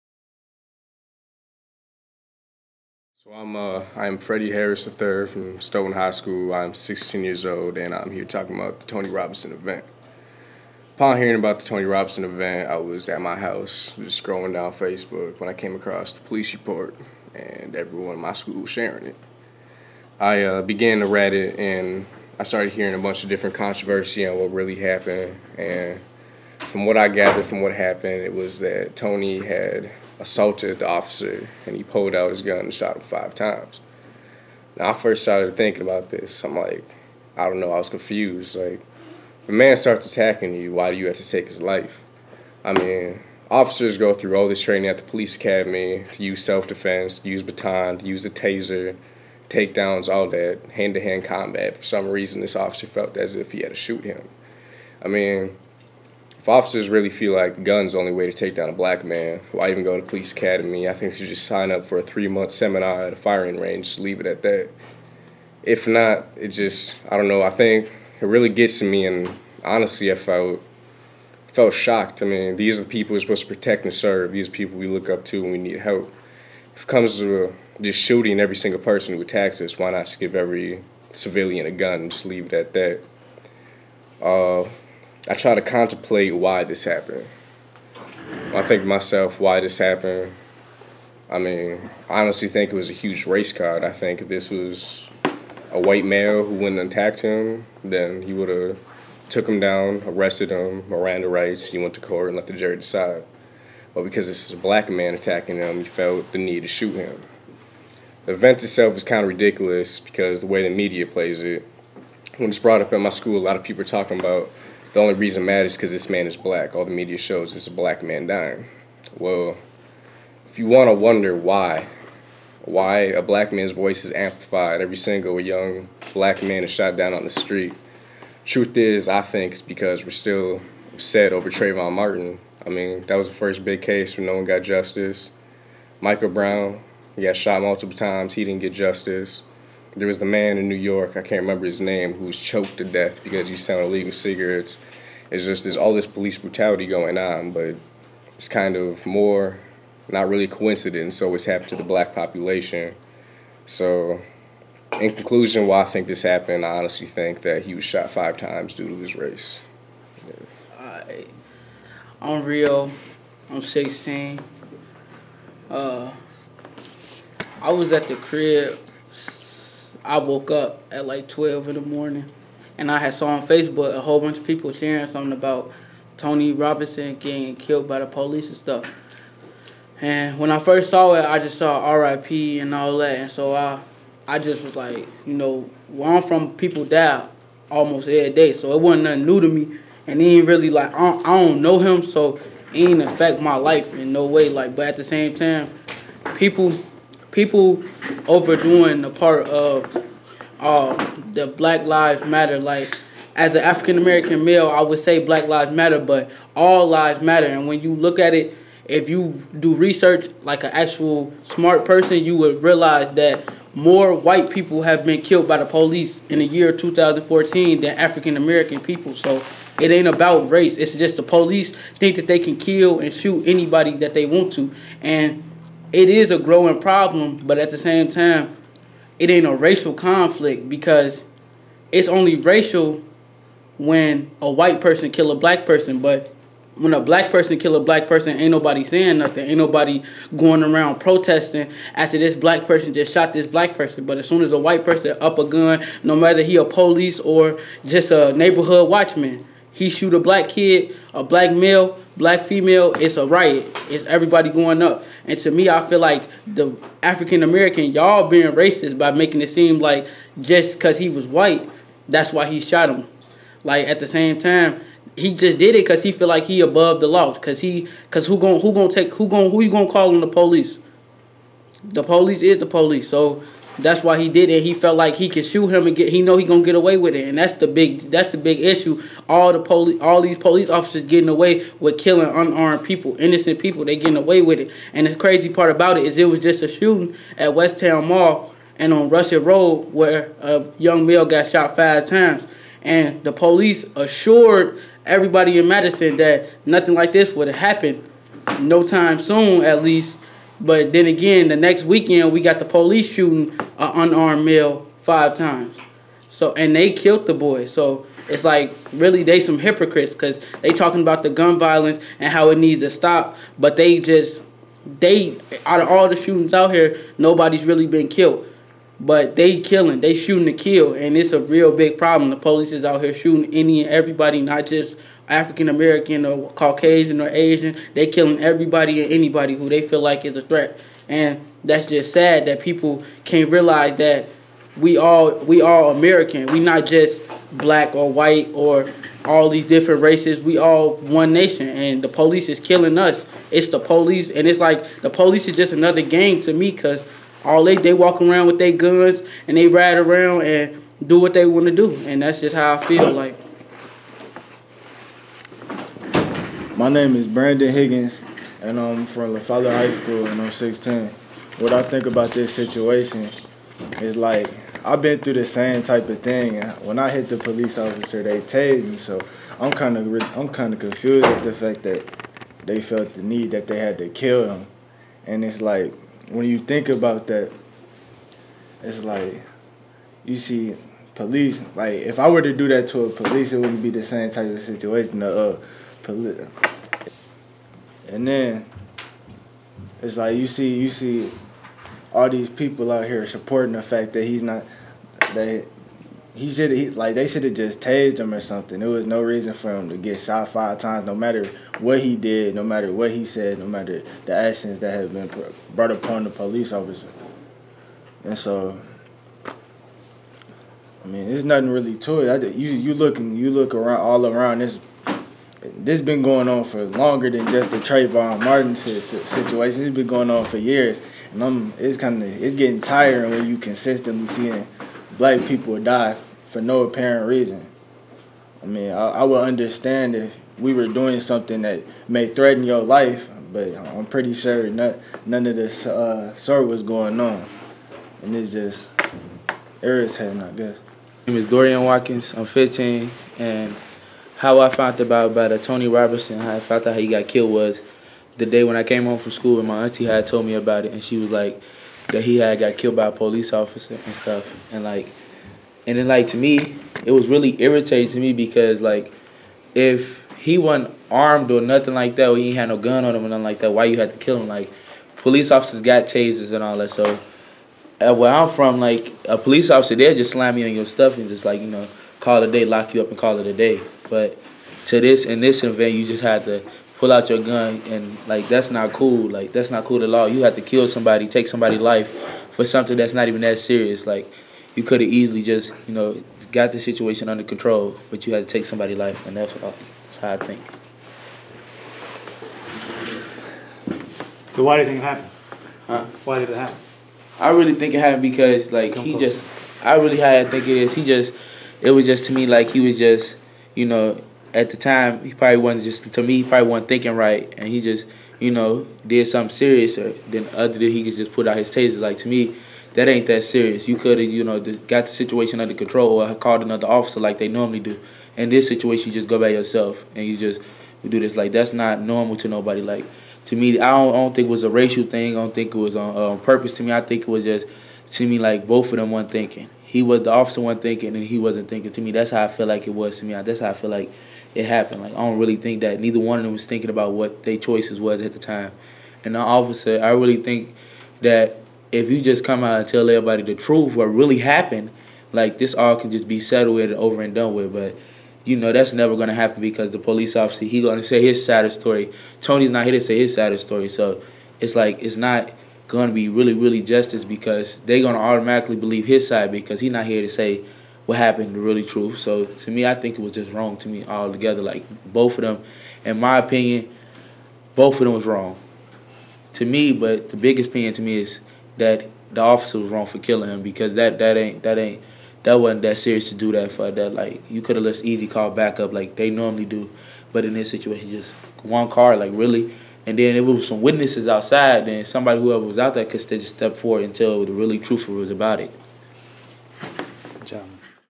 Briarpatch Reactions 720p
Several boys attending a Briarpatch program had a chance to respond to the Tony Robinson tragedy here in Madison. Briarpatch is a non-profit youth services organization that helps area youth find their way, especially when confronted with trauma, homelessness, abuse, or other difficulties.